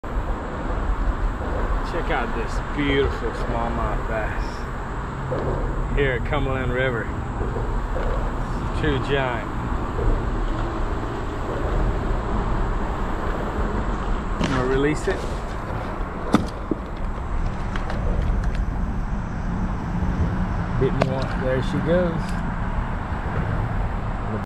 Cumberland river small mouth bass!riverfishing bassfishing riverlife fishing subscribe and like!
Fishing for smallies over and arround rocks and lay downs! Cumberland river is a great place to enjoy small mouth bass fishing!